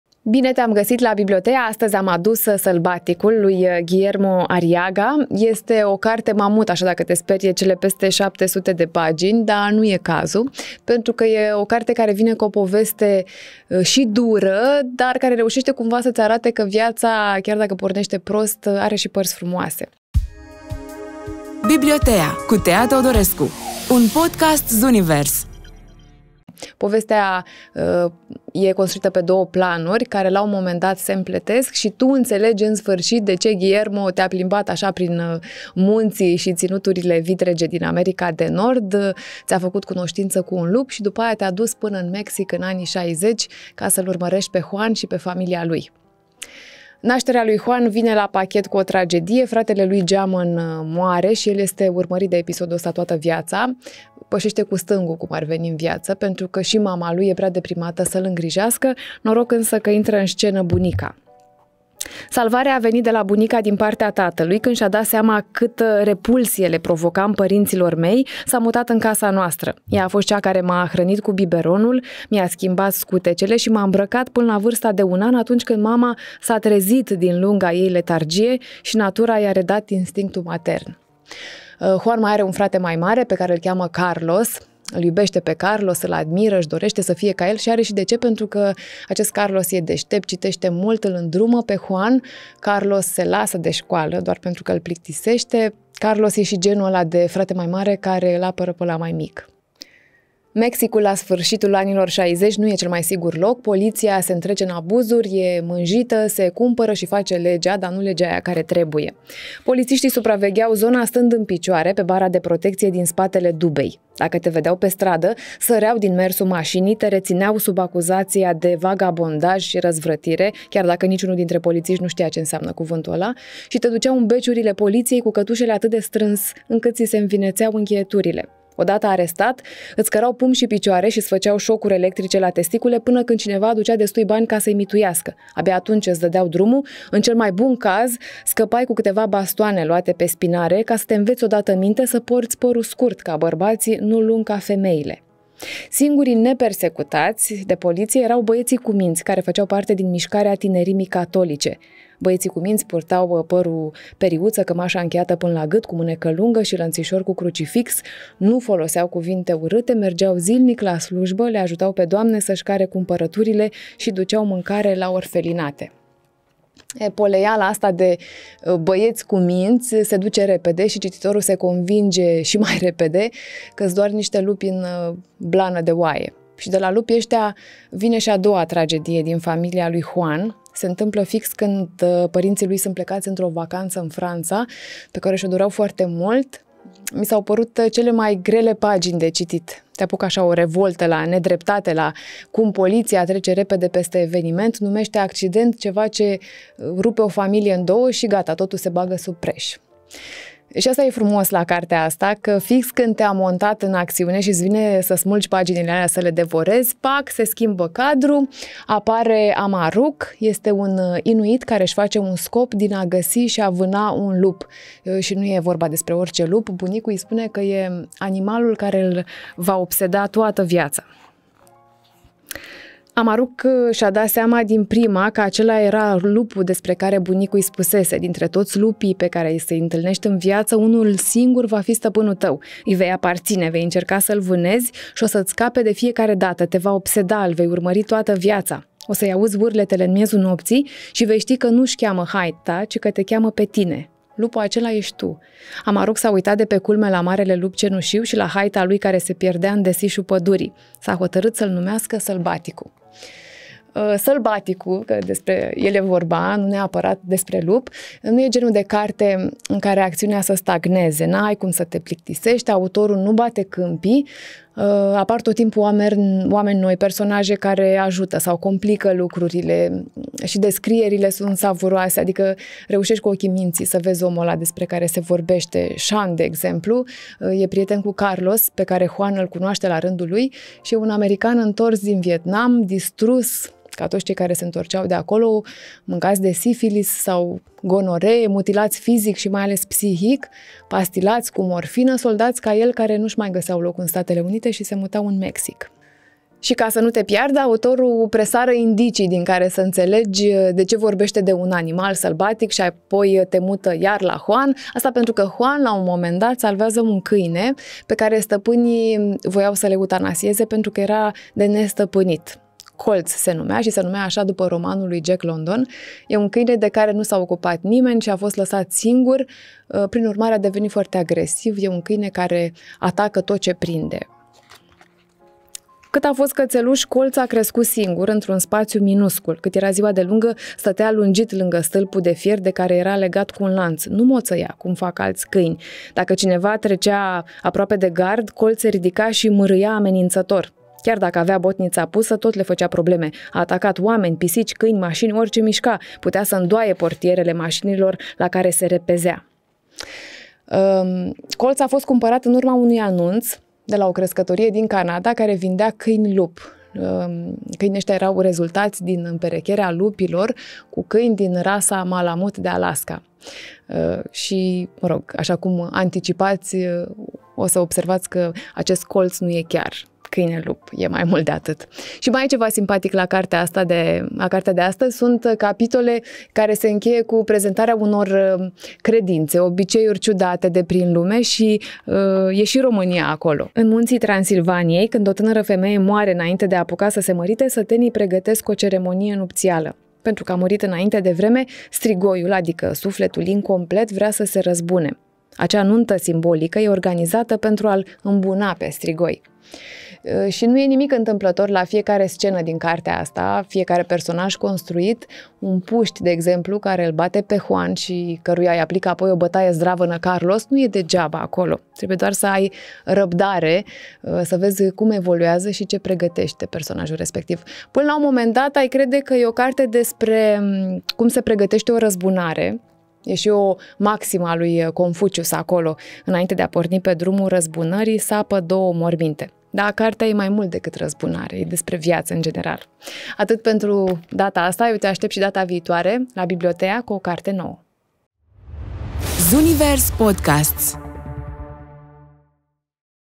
„SĂLBATICUL” - GUILLERMO ARRIAGA | BiblioTEA cu Tea Teodorescu 39
Tea ne aduce astăzi cartea „Sălbaticul” de Guillermo Arriaga, cel mai ambițios roman al scriitorului. Poate îl cunoașteți pentru contribuțiile cinematografice în: „Babel”, „21 grams”, „Los amores perros” sau „Burning Plain”. Autorul pleacă de la premisa că „obligația unui romancier este de a descoperi sensul morții pentru a celebra viața”.

Povestea pe care se bazează cartea pleacă de la două fire: cel al lui Juan Guillermo, un adolescent mexican care își pierde, în decurs de câteva luni, întreaga familie și ajunge să devină obsedat de răzbunare, fiind măcinat de vina supraviețuitorului; cel al lui Amaruq, inuitul care își pierde viața în vânătoarea lupului Nujuaqtutuq, animalul care ar purta spiritul bunicului său. La un moment dat, cele două povești se vor întrepătrunde.

Descoperă un roman despre iubire, ură, răzbunare și sacrificiu.